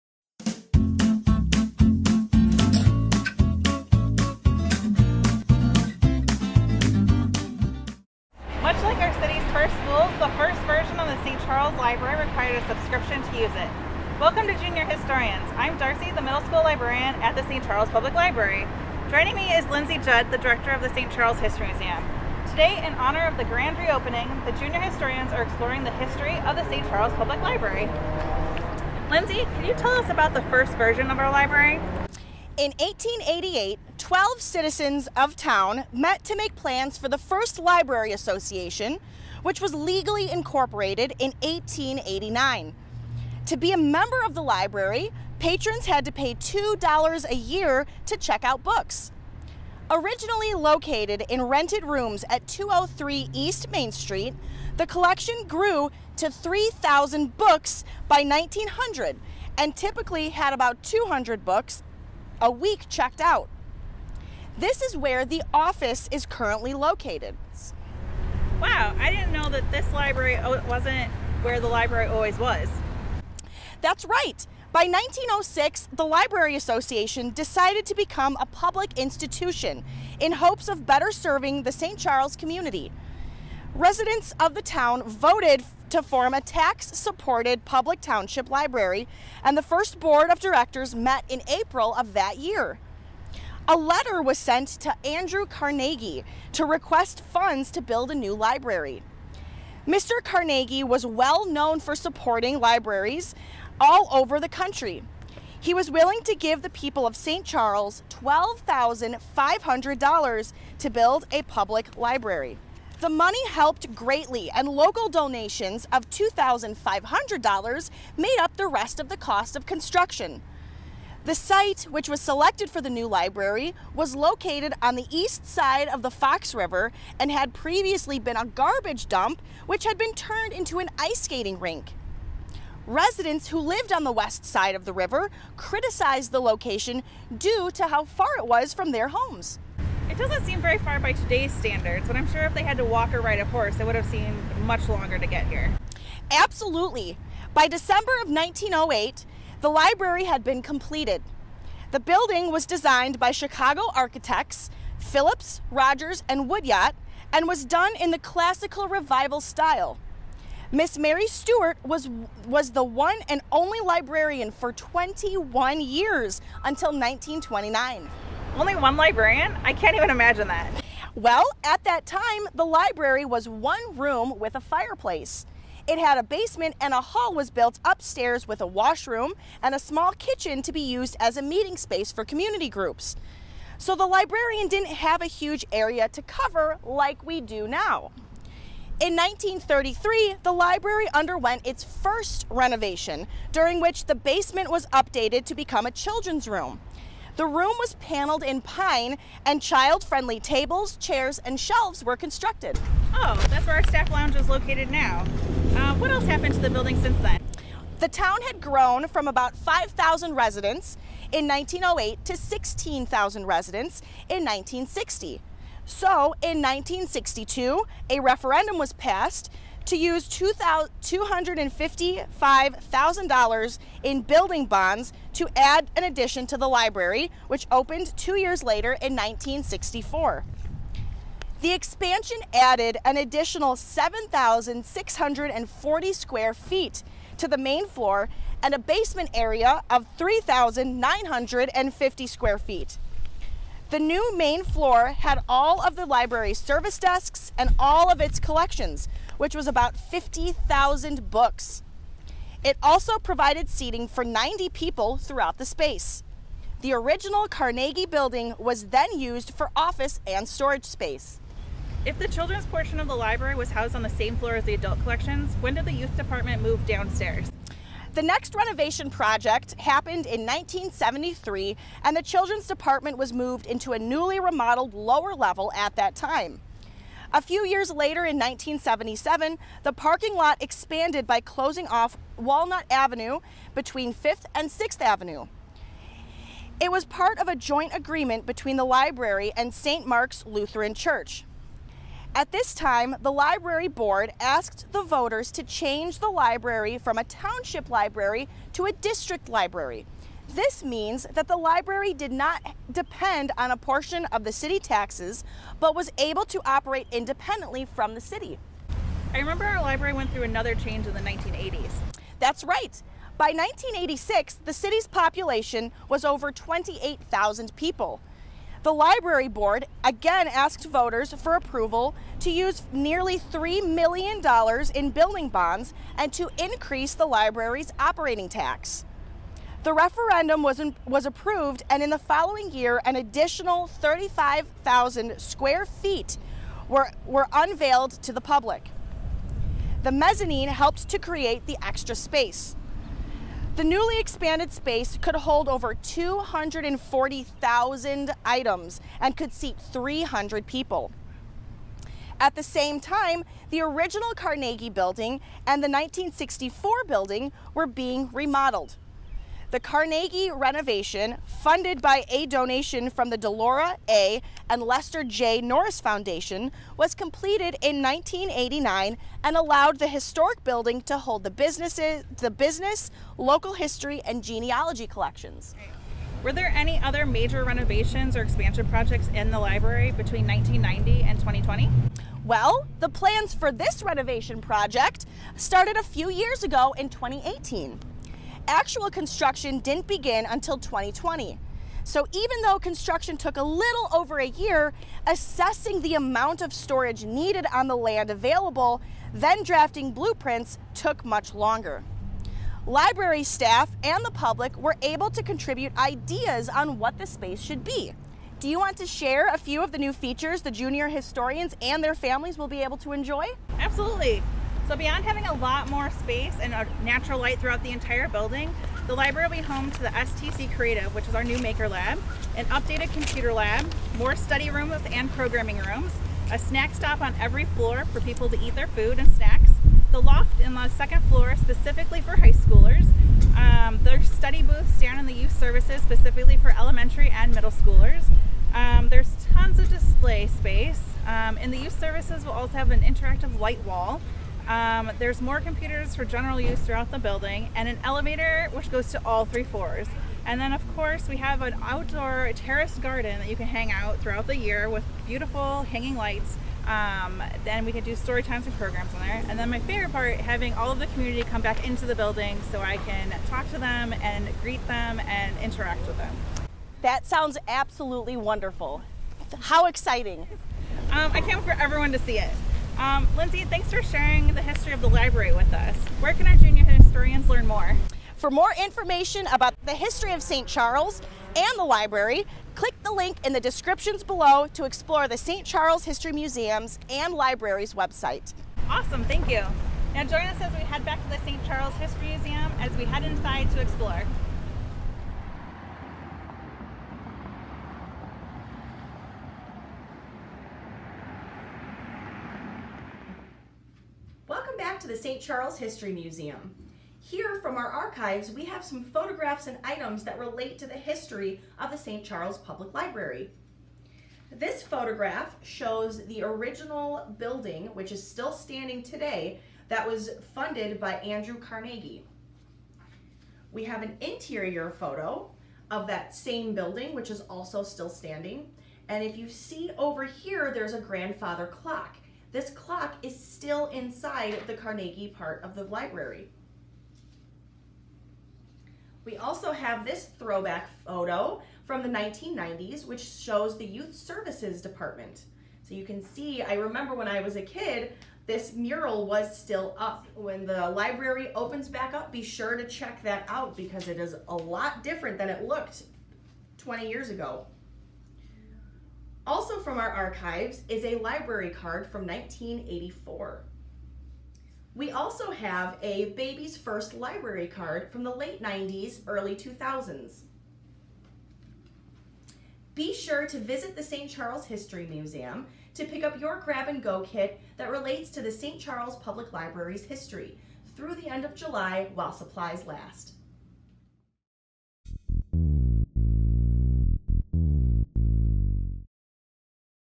Junior Historians: St. Charles Public Library District, IL
Pick up a Grab and Go Kit, related to the St. Charles Public Library, from the St. Charles History Museum starting July 9, 2021 through the end of the month, while supplies last.